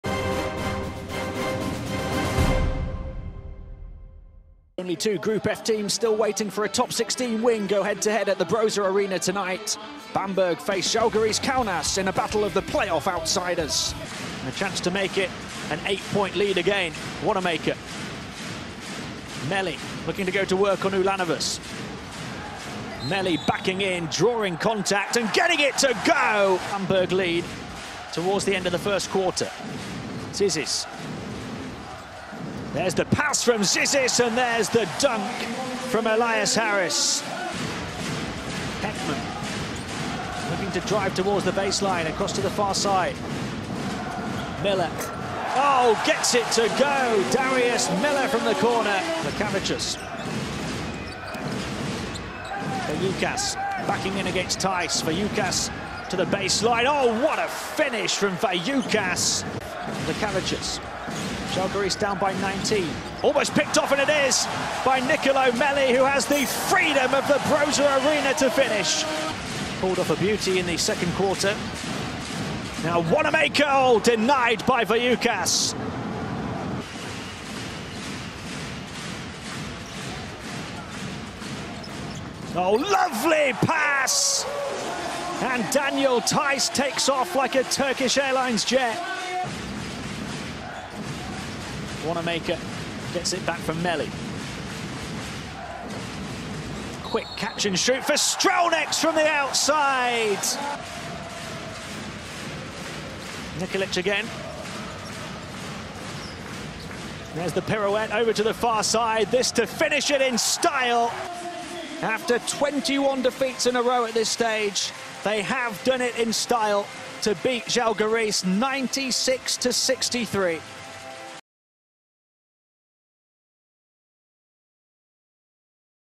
Highlights: Brose Baskets Bamberg-Zalgiris Kaunas
The wait is finally over as Brose Baskets Bamberg broke through in dominant fashion to its first-ever Top 16 victory by racing past Zalgiris Kaunas 96-63 at home on Friday. Bamberg had lost its previous 21 games in the Top 16 and all eight games it had played in the past with Zalgiris.